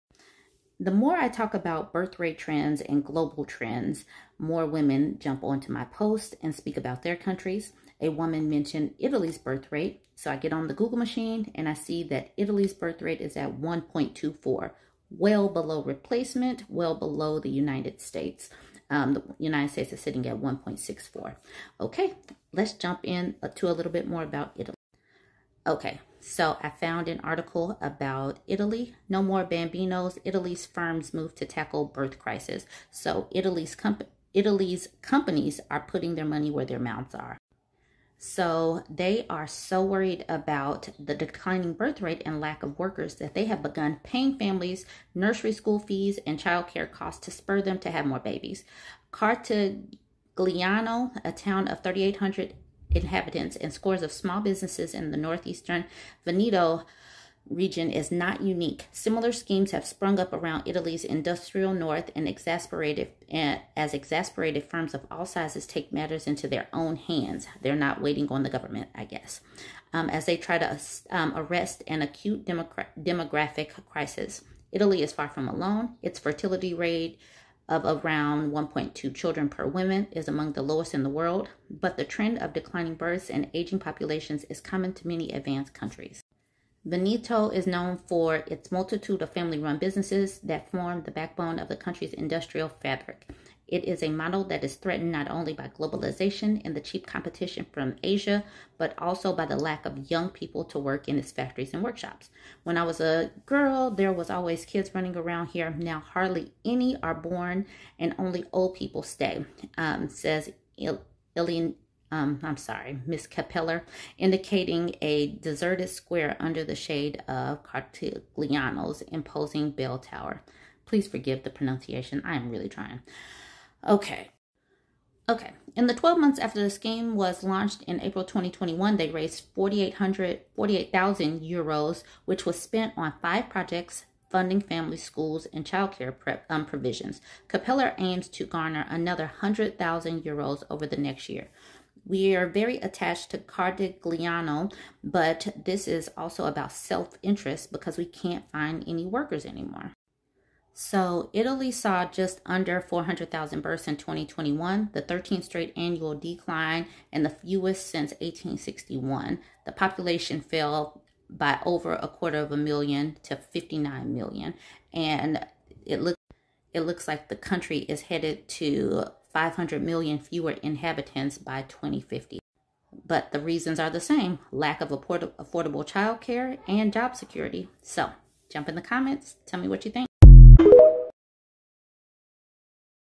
Italy's Birth Dearth and What Some Employers are Doing About It
Let's look at Italy's birth rate decline.